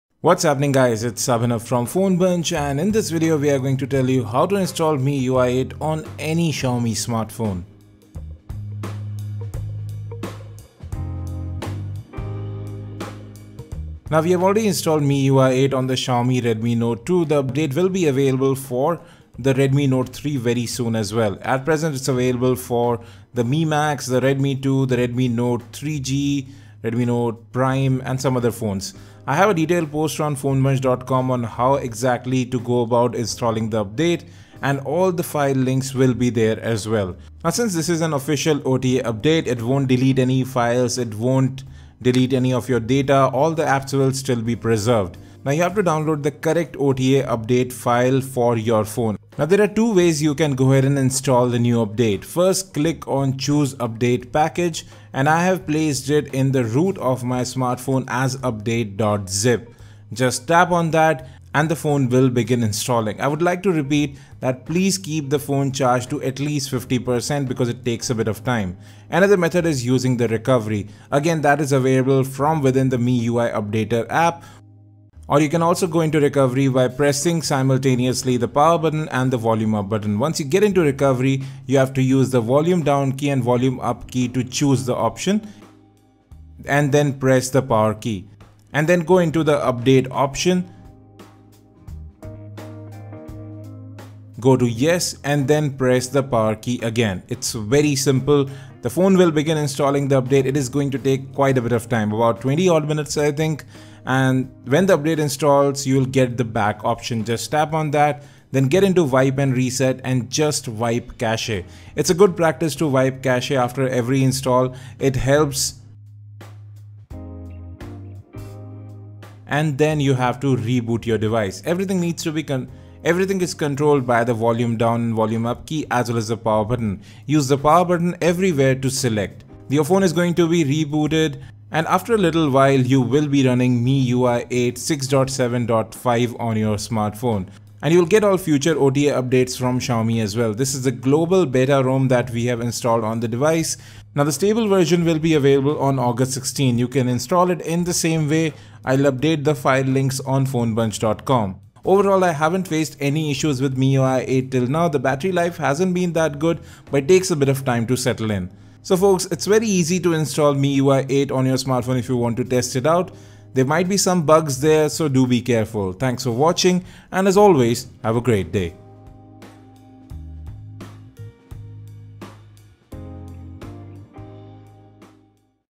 How to Install MIUI 8 on Redmi Note 3, Mi 4i, Mi Max, Redmi 2 and Others
Tutorial to install MIUI 8 on Redmi Note 3, Mi 4i, Redmi 2, Mi max and other smartphones.

Note: Backup data before proceeding, on some phones including Redmi Note 3, MIUI 8 update is removing data and formatting device.